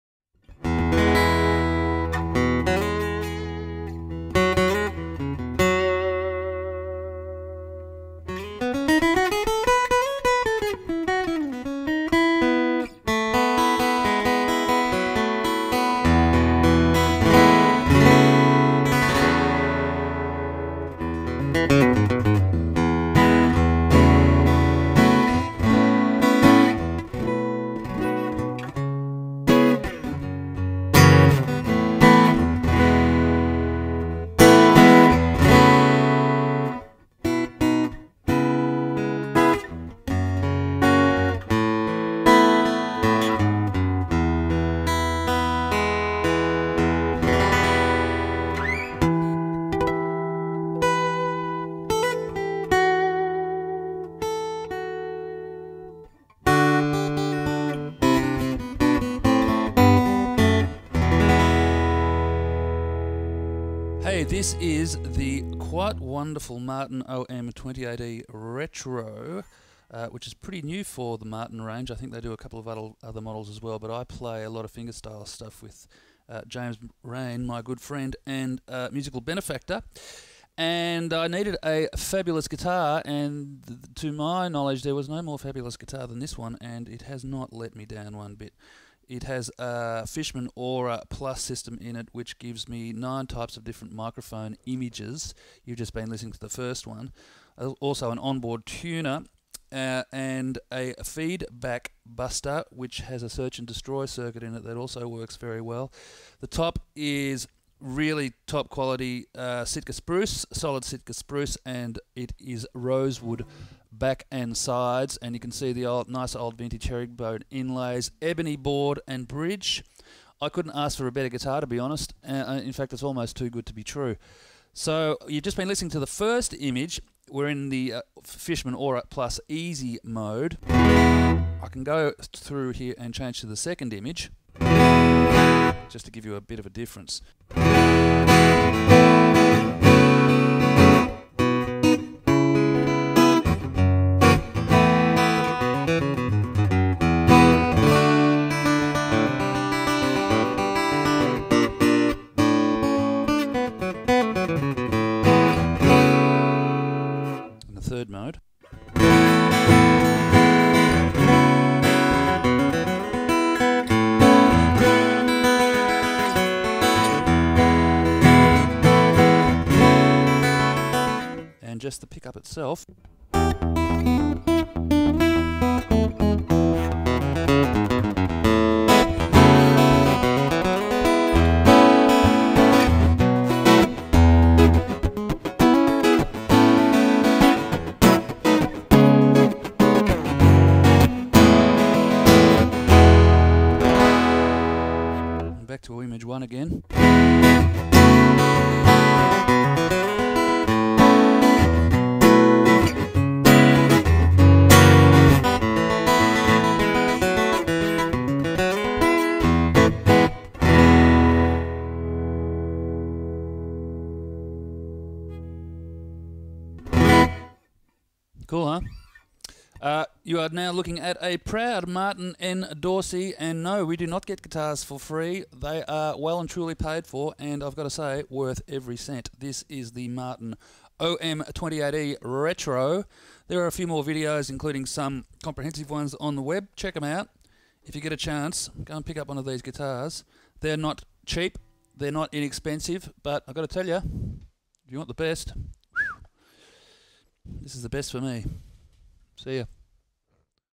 C.F. Martin & Co., OM-28E Retro Acoustic Guitar.
A quick look at the mighty C.F. Martin & Co., OM-28E Retro, a guitar that I have chosen to accompany me on my acoustic journeys around Australia and the world. This video covers only the Aura+ side of the instrument. A further video where I mic the guitar may be uploaded at a future date. I will be using the instrument primarily on stage and in the studio hence my choice to show off its to-the-board qualities.

This video was not commissioned by C.F. Martin or their (excellent) Australian distributor, Electric Factory; rather, it was made on the back of pure enthusiasm for a fantastic instrument, an instrument which I was more than happy to pay for. However, if you'd like to see more Martin demos on this channel, let me know and I'll see what I can arrange.

Cheers,

Brett K.